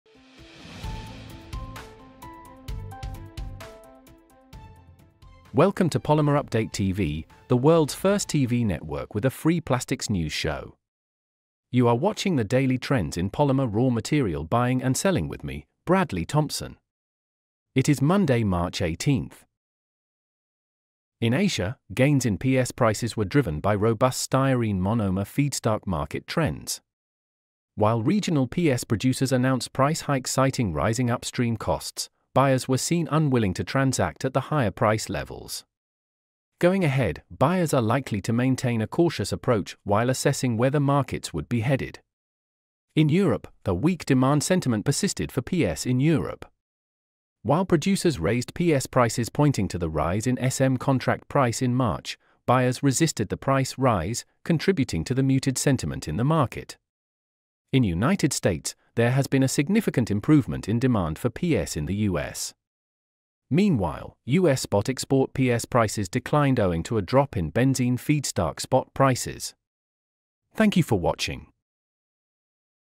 Polymer News: Global Polystyrene Market Outlook
Today's polymer news and price changes in the global petrochemical industry with Bradley Thomson.